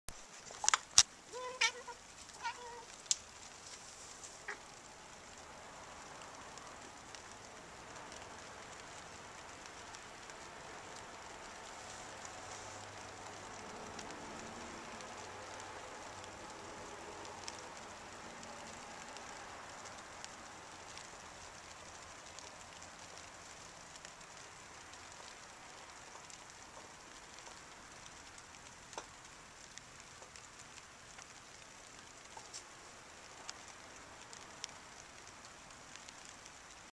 Humboldt Humming Bird Moth - White Lined Sphinx Moth - Hyles lineata
Stepped out my back porch to find this White Lined Sphinx Moth (Hyles lineata) feeding off some flowers in my back yard.  Managed to grab the flip and film this clip before the camera's batteries died. Listen to them wings roar!